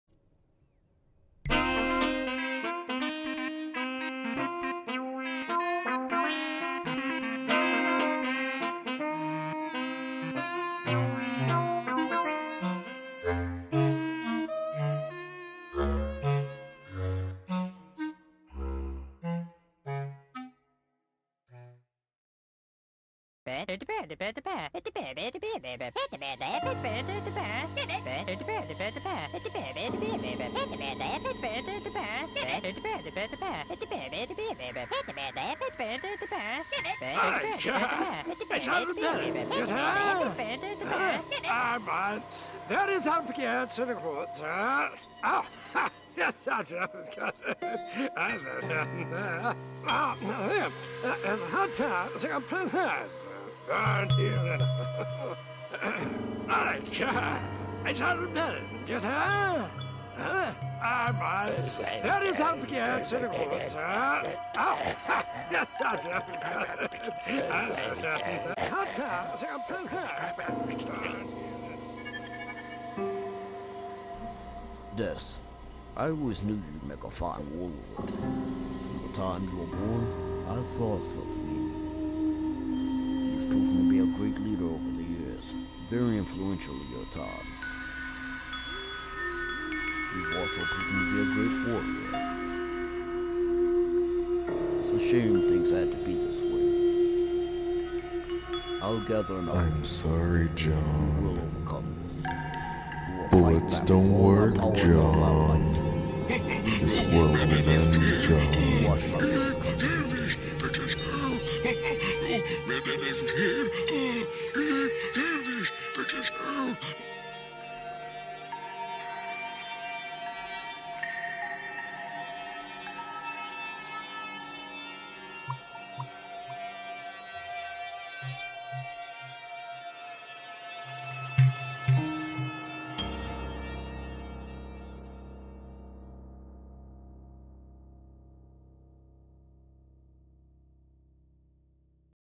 Nightmare Music
A surreal Nightmareish Pice of Music I Made Using Dreams on PS4 by putting and mixing Different Erie Sonds and Voices.

Scary Not For Kids.